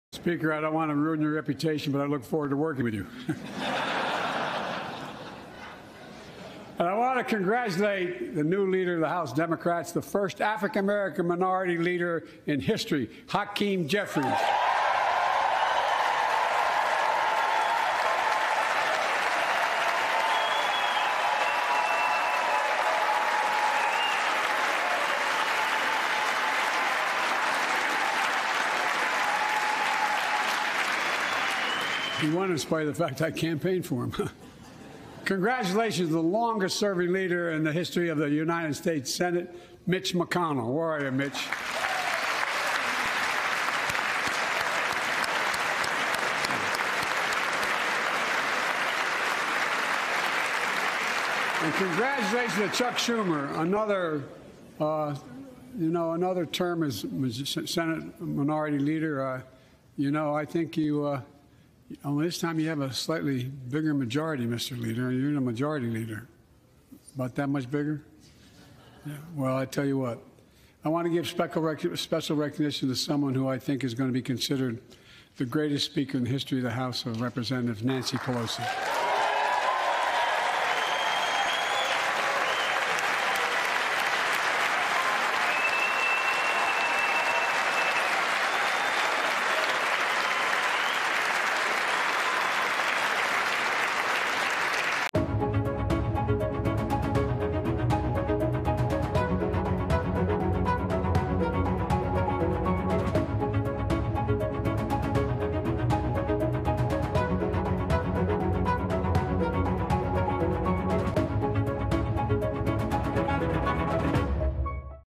‘Greatest Speaker in the history of the House of Representatives’: Biden thanks Nancy Pelosi
President Biden opened his State of the Union speech with special recognition to congressional leaders, and also former House Speaker Nancy Pelosi, who he called "the greatest Speaker in the history of the House of Representatives."
President Biden delivers 2023 State of the Union address. Live updates